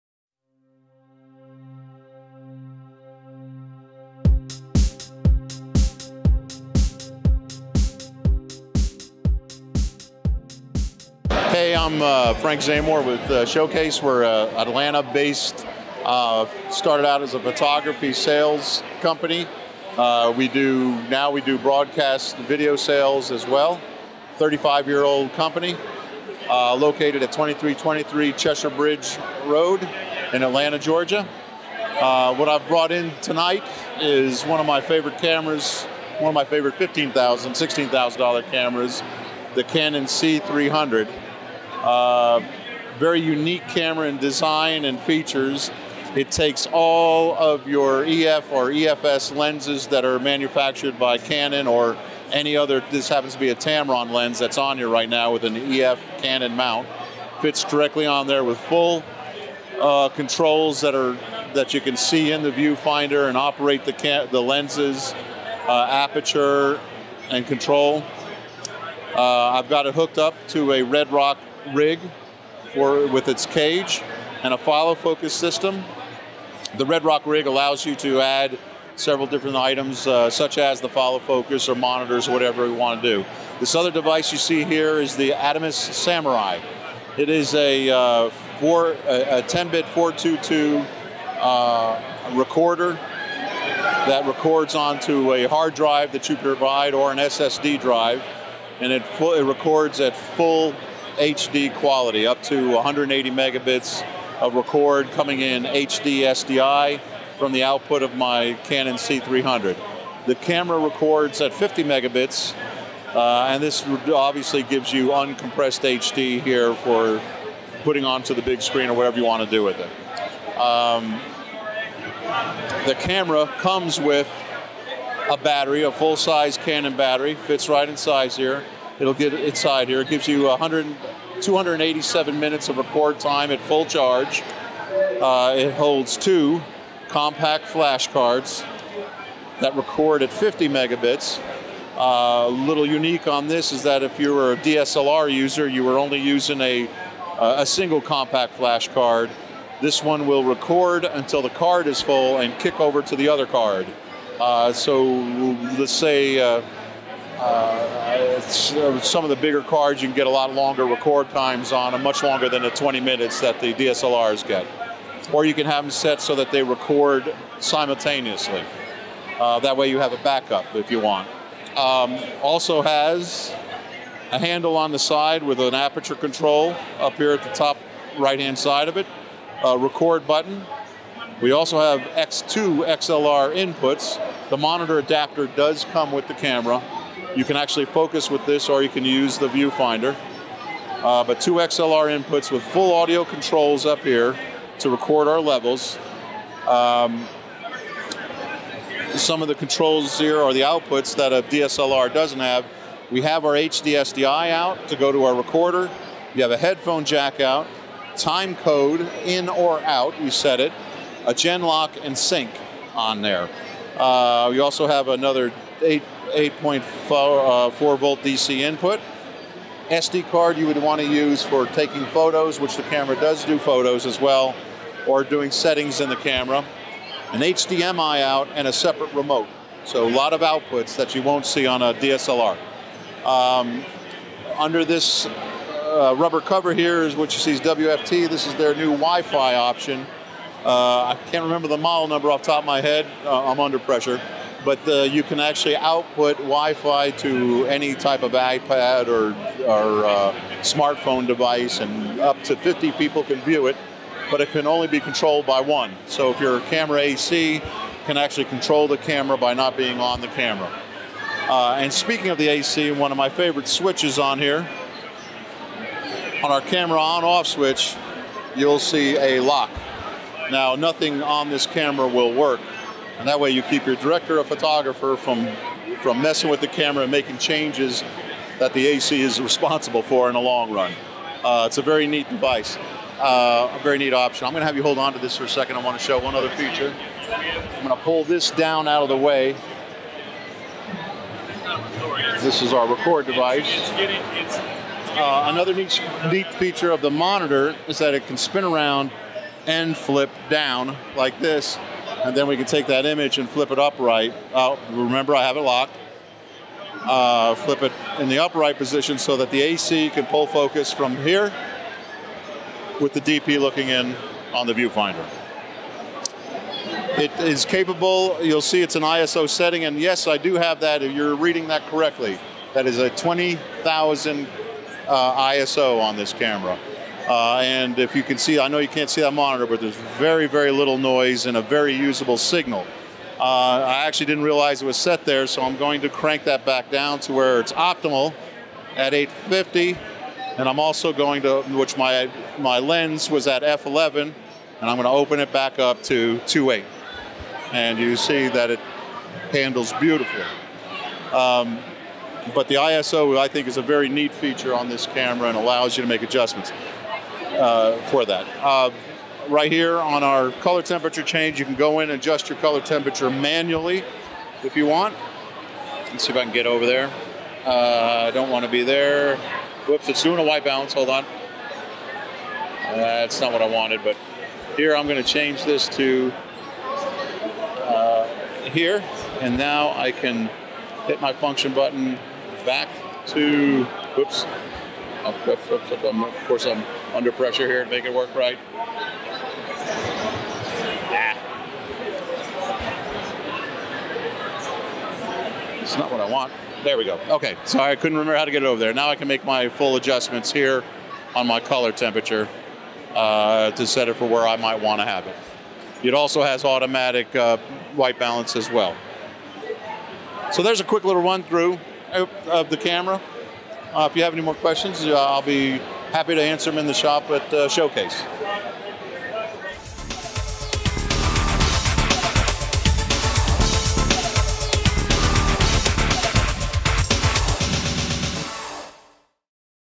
Canon C300 In Depth presentation - Get Connected
Canon C300 In Depth presentation - Get Connected from The Lounge Magazine.
At Atlanta's monthly Get Connected event we meet with ShowCase Photo  Videoapos's Frank Zamor, as he shows off the Canon C300.

The Canon C300 Details:
-8.29 million effective pixels and has a pixel size that is larger than that for conventional professional camcorders
-Featuring a unique Super 35mm Canon CMOS sensor
-Enabling greater light-gathering capabilities for enhanced sensitivity and reduced noise.
-The sensor reads Full HD (1920 x 1080 pixels) video signals for each of the three RGB primary colors, decreasing the incidence of moire while realizing high resolution with 1,000 horizontal TV lines.
-Supported by a heightened signal read-out speed
-The CMOS sensor reduces rolling shutter skews
-A phenomenon prevalent with CMOS sensors in which fast-moving subjects may appear diagonally distorted
-In addition to MPEG-2 Full HD (MPEG2 422@HL compliant) compression, the EOS C300/C300 PL employs 4:2:2 color sampling for high-resolution performance that minimizes the appearance of "jaggies" at chroma edges.
-Additionally, with a maximum recording rate of 50 Mbps, the camcorder supports the recording of high-quality video
-Revolutionary Canon DIGIC DV III Image Processor facilitates high-precision gamma processing and smooth gradation expression
-Compact, modular and compatible with Canons existing line-up of EF and new EF Cinema lenses as well as accessories from third parties
-Designed to please cinematographers for any production large and small, on location or in the studio, from shoestring to Hollywood budgets.
-The EOS C300 has a Canon EF lens mount and is compatible with all of Canons new EF Cinema lenses and current EF lenses, including compact primes, super telephotos, specialty tilt-shift, macro and fisheye - over 60 in all.
-Its MPEG-2 codec is an internationally recognized standard compatible with major NLE applications while additional features such as Canon Log ensure seamless integration with existing production workflows, making post-production easy.
-The camcorderapos;s video and audio recording file format adopts the industry-standard MXF (Material eXchange Format), an open source file format ideally suited for non-linear editing systems. Take a look as to why this is such an amazing camera.